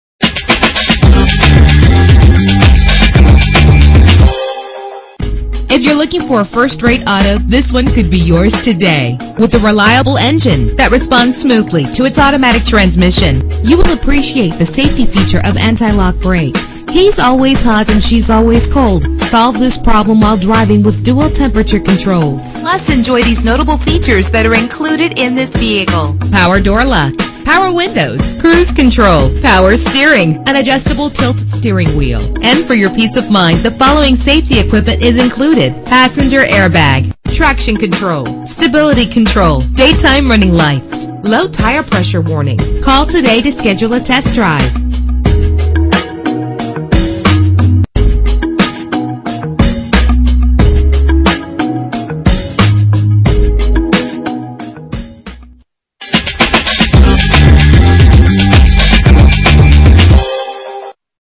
2006 Lexus RX - Northlake IL
Year: 2006
Make: Lexus
Model: RX
Trim: RX 330 Sport Utility 4D
Engine: 6 cylinder 3.3 Liter
Transmission:
Color: Blue
Mileage: 53165
Address:
401-11 East North Ave